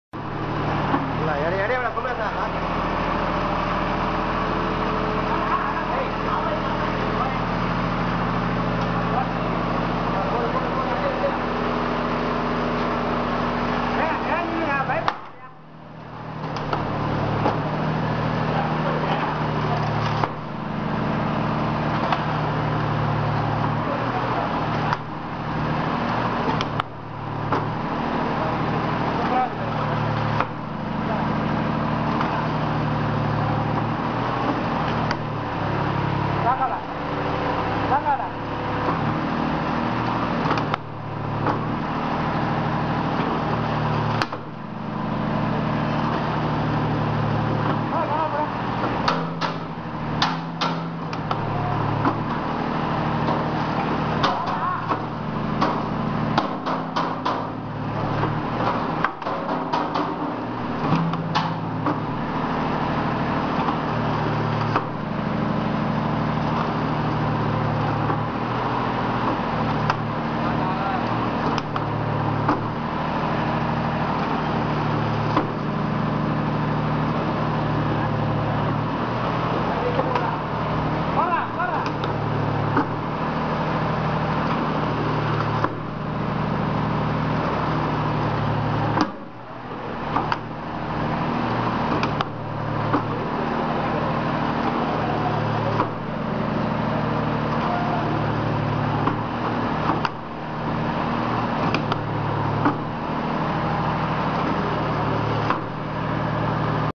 Auram Press 6000 - automatic compressed earth block press
Prototype of the Auram Press 6000 - Aureka's fully automatic compressed earth block press.